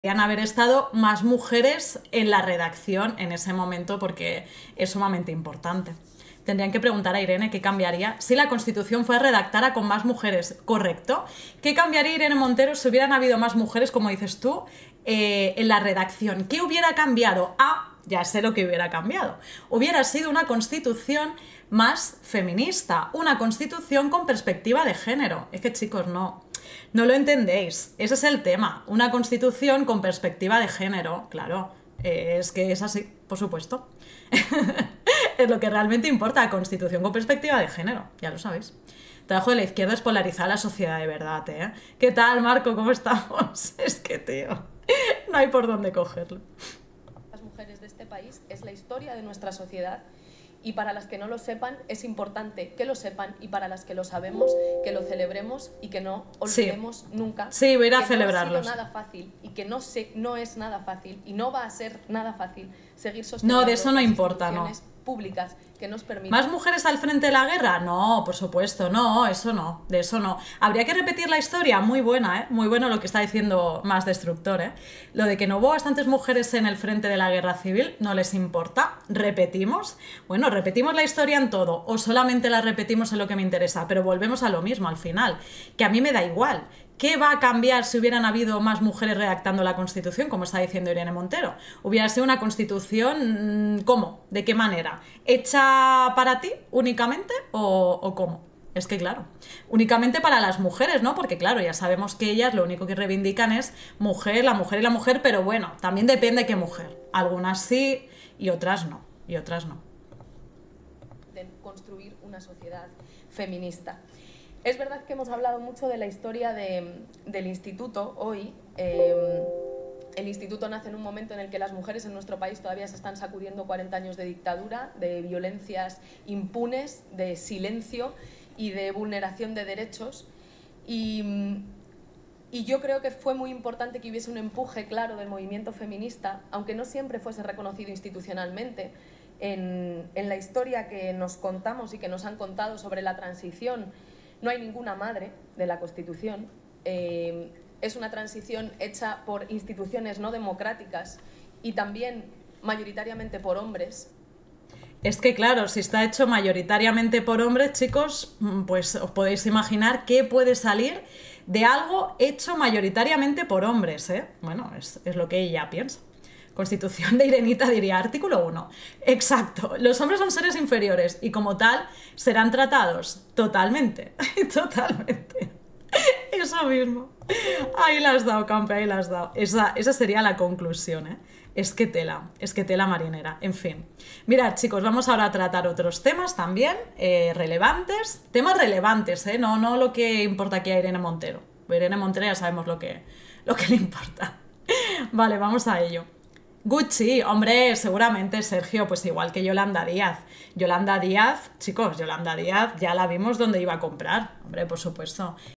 ​​IRENE MONTERO sigue LLORANDO y JUSTIFICANDO su MINISTERIO de DESIGUALDAD
​​IRENE MONTERO CRITICA la falta de MUJERES en la redacción de la CONSTITUCIÓN. Es todo SURREALISTA​